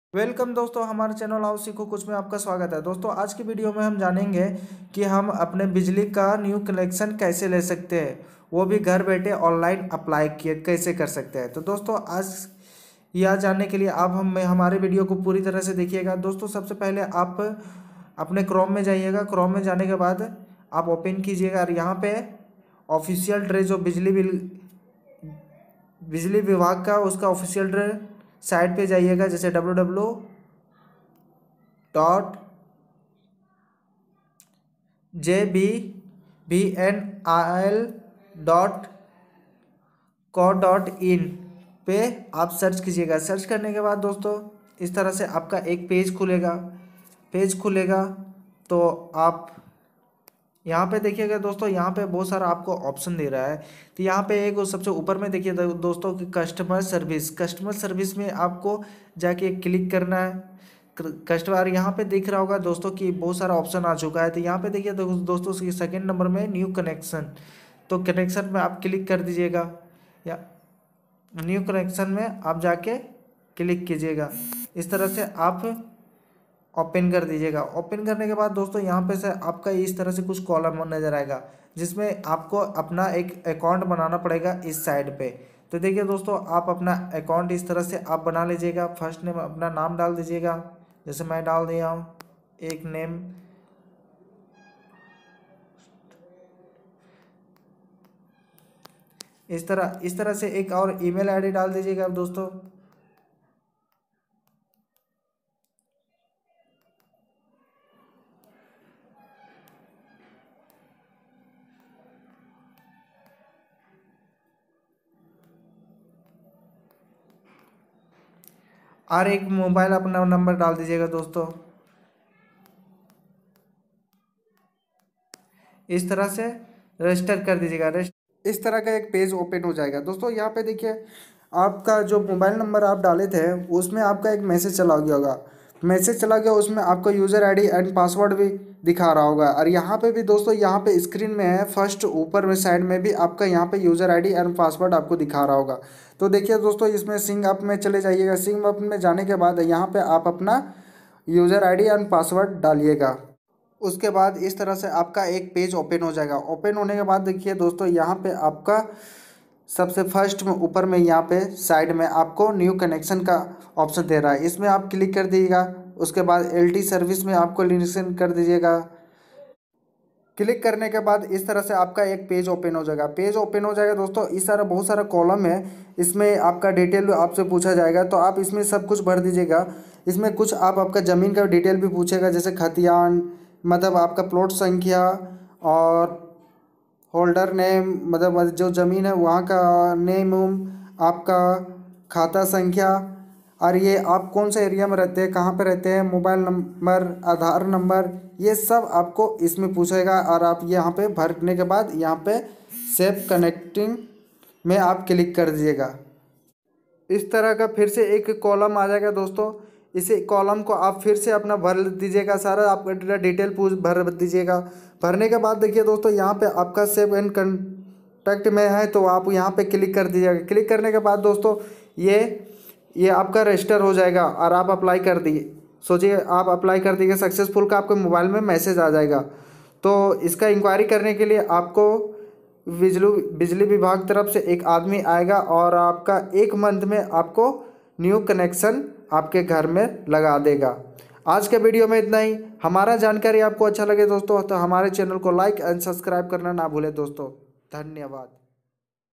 Bijli Online New Connection Kare 1 minat mein (jharkhand)
Aap apne ghar bethe Online bijli ka New Connection kese le sakte hai aap es video mein janenge .